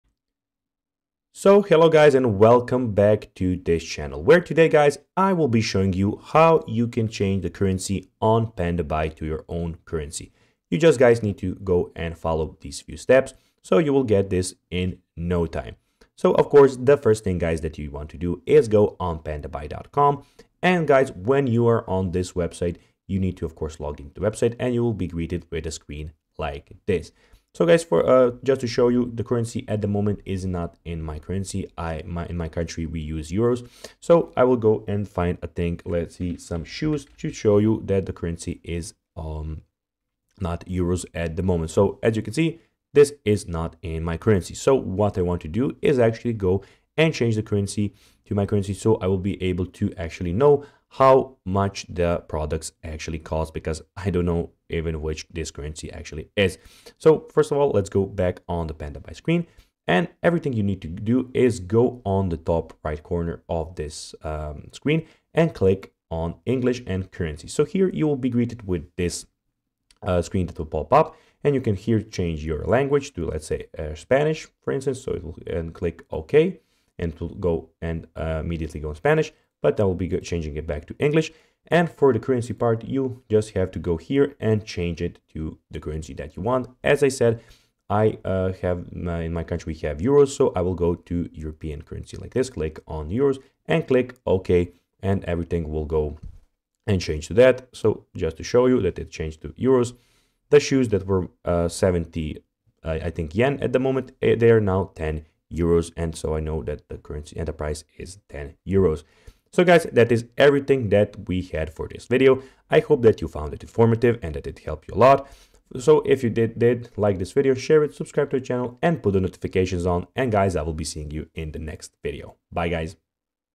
How To Change Currency On Pandabuy (Easiest Way)
In this video I will show you How To Change Currency On Pandabuy. It would be good if you watch the video until the end so that you don't miss important steps.